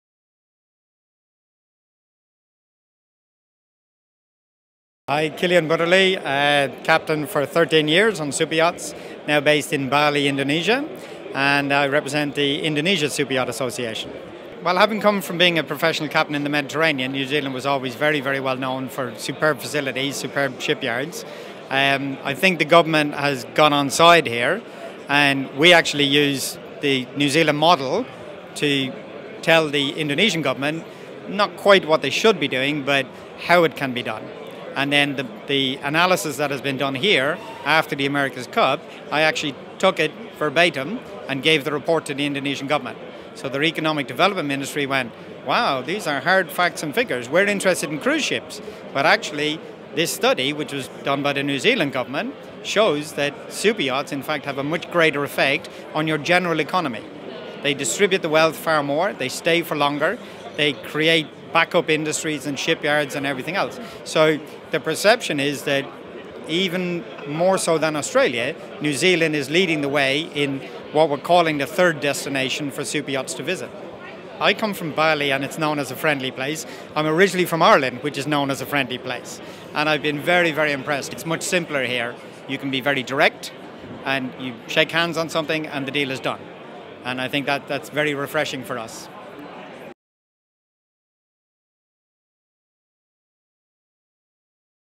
Cilian Budarlaigh: Interview on Indonesian Superyachts and the NZ Marine Economy
Cilian Budarlaigh is a captain of superyachts and represents the Indonesian Superyacht Association.

Budarlaigh talked with InvestAuckland as part of the Auckland International Boat Show about the New Zealand marine model and how superyachts have a better effect on the general economy, from distributing wealth to creating backup industries.